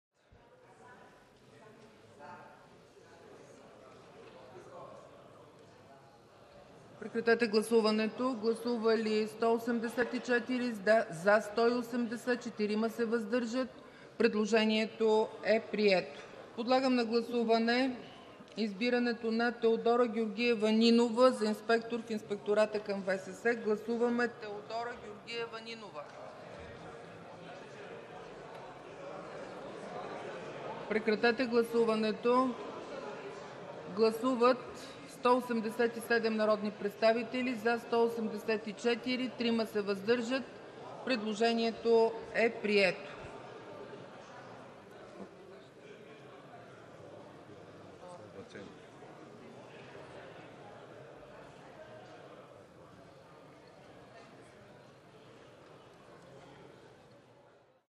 Депутатите избраха новите членове на Инспектората към ВСС
С квалифицирано мнозинство от две трети днес депутатите избраха 10-те инспектора към Инспектората на ВСС.

Общо кандидатурите бяха 19 и всички бяха изслушани на заседание на Комисията по правни въпроси на 20 януари т.г. Новите инспектори са: Александър Мумджиев, Генади Георгиев, Игнат Георгиев, Лидия Стоянова, Любка Кабзималска, Любомир Крумов, Мария Нейкова, Стефка Мулячка, Теодора Нинова, Юрий Кръстев. /БГНЕС